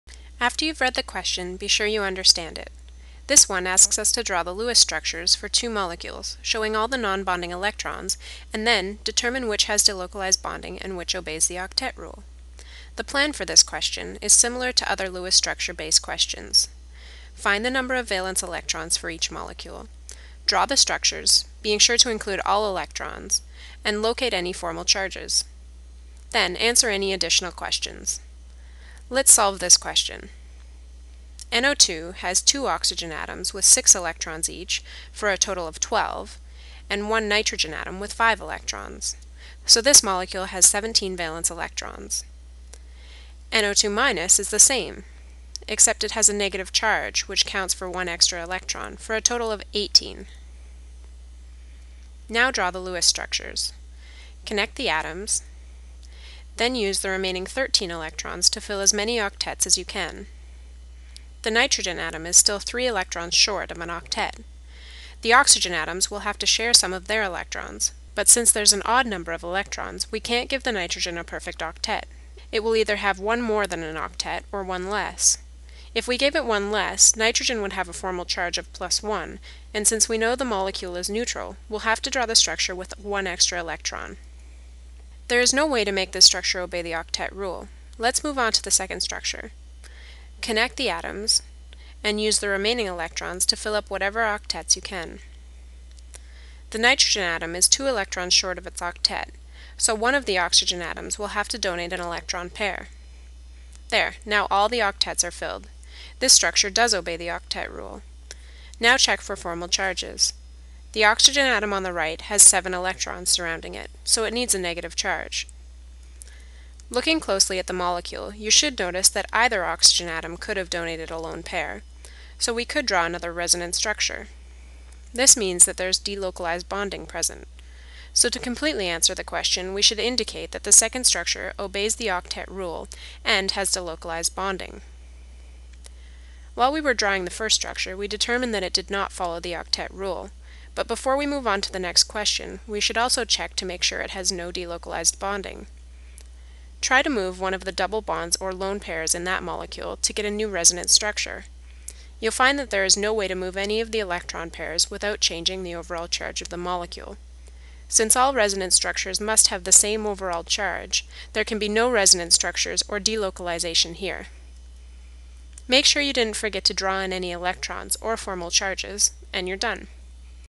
RPSC 3-5
A short video demonstrating the use of a standard problem-solving strategy:
READ-PLAN-SOLVE-CHECK.
Example is from the Chemistry 101 course at the University of Victoria, BC, Canada.
Content by Scott McIndoe and Krista Vikse.